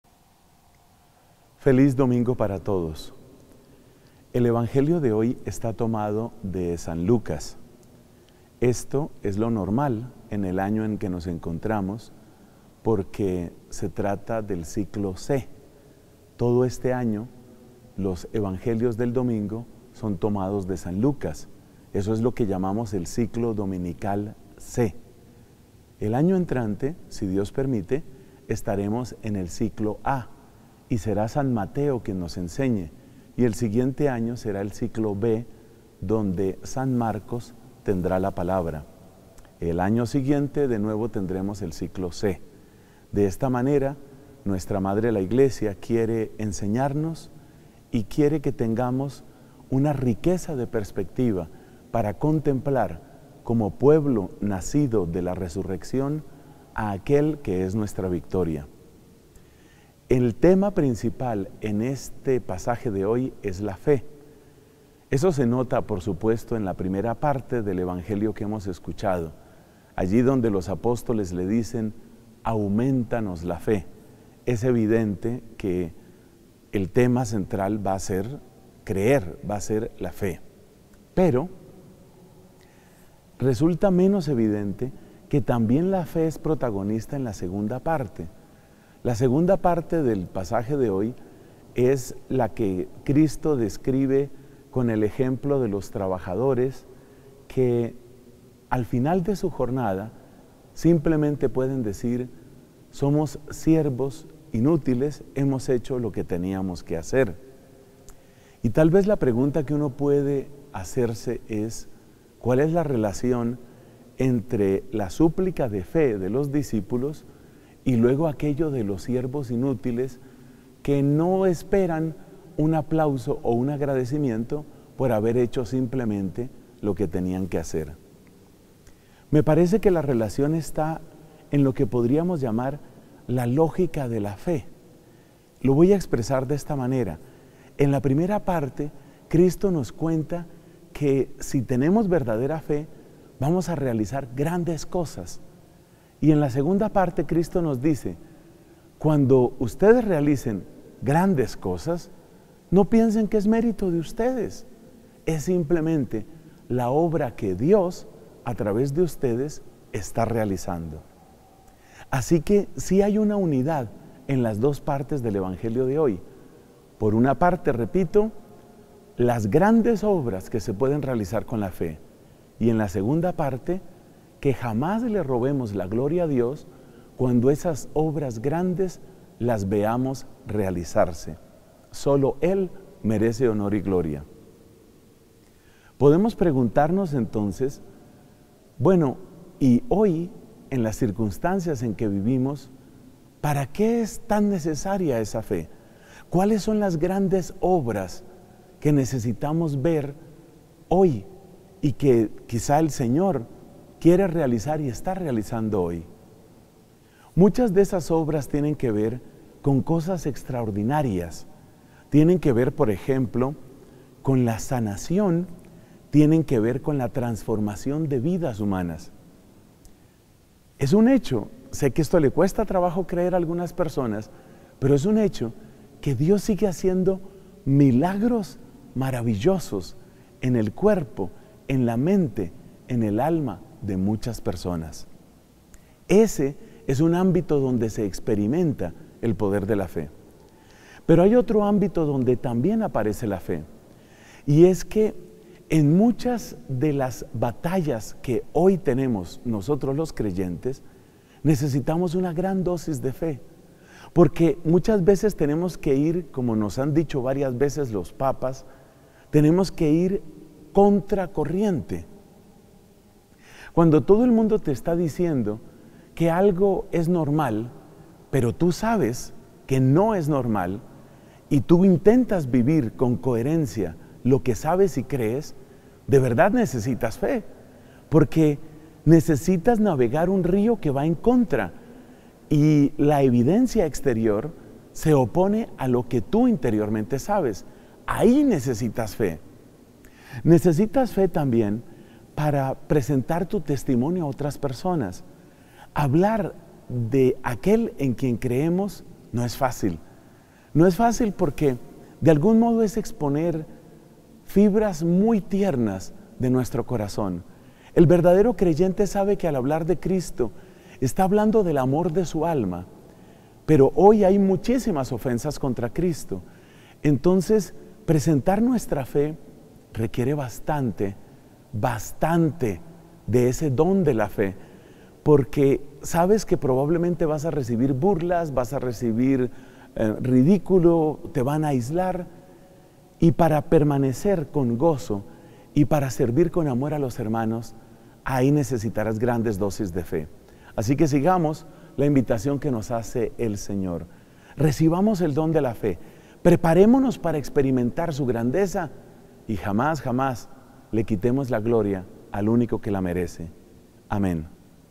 LA GRACIA del Domingo 2 de Octubre de 2016
DOMINGO XXVII DEL TIEMPO ORDINARIO, CICLO C
Recibamos el don de la fe, preparémonos para experimentar su grandeza en las obras extraordinarias que realicemos, sin quitarle jamás la gloria al Único que la merece.